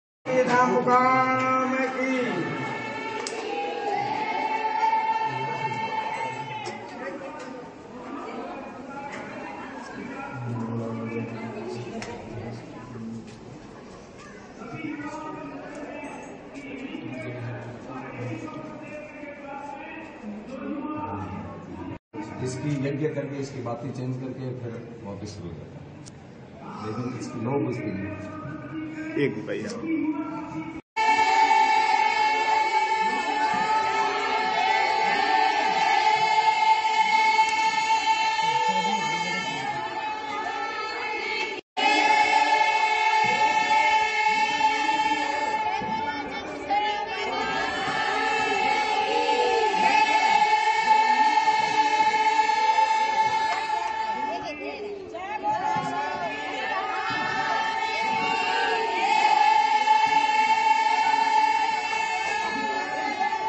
Barmer:चुनावी प्रचार शुरू करने से पहले CM भजनलाल ने जम्भेश्वर मंदिर में पूजा की
मुख्यमंत्री भंजनलाल शर्मा ने बाड़मेर के जम्भेश्वर मंदिर में पूजा-अर्चना की।